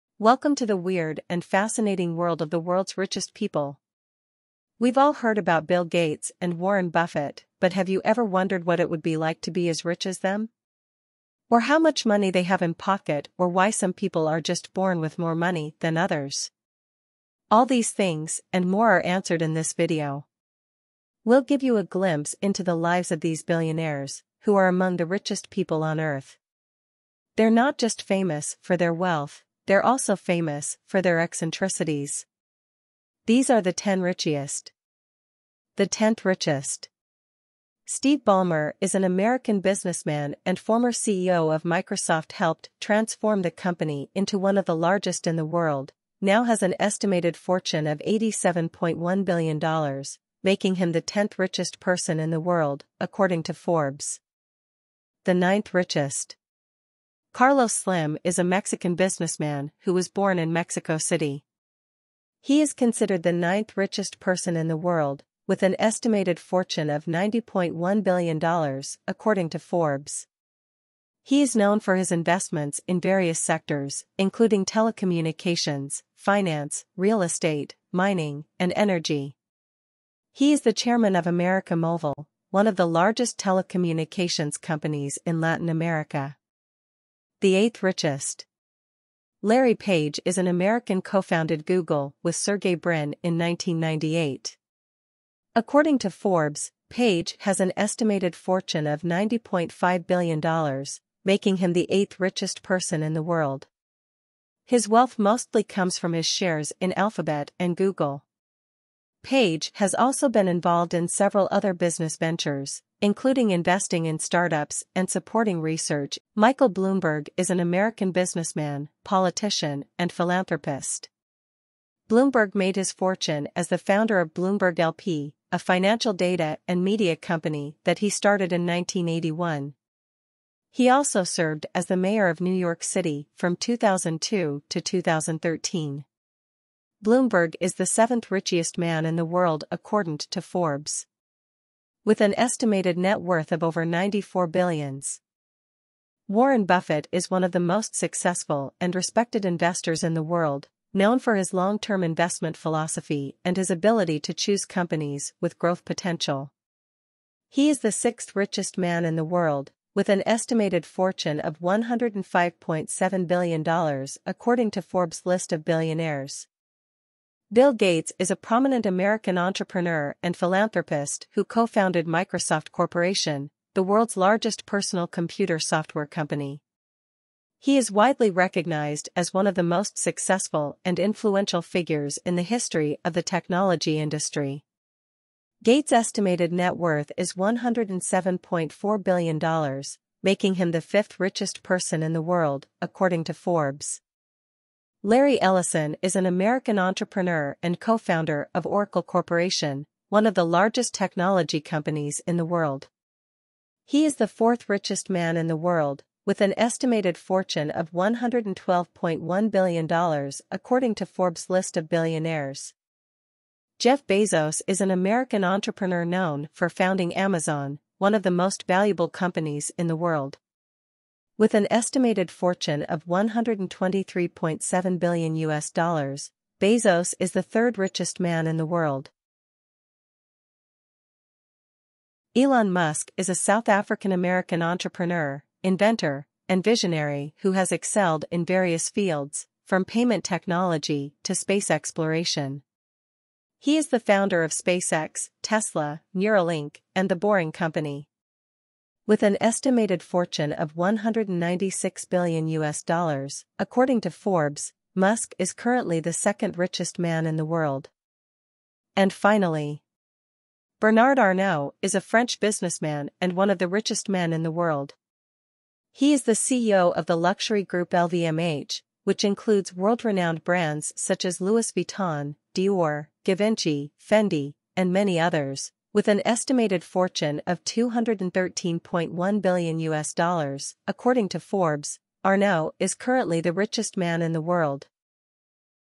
Top 10 Richiest person list
We've all heard about Bill Gates and Warren Buffett, but have you ever wondered what it would be like to be as rich as them? Or how much money they have in pocket Or why some people are just born with more money than others?

All these things and more are answered in this video. We'll give you a glimpse into the lives of these billionaires, who are among the richest people on Earth. They're not just famous for their wealth; they're also famous for their eccentricities. These 10 rich people have some interesting habits that make them stand out from other billionaires.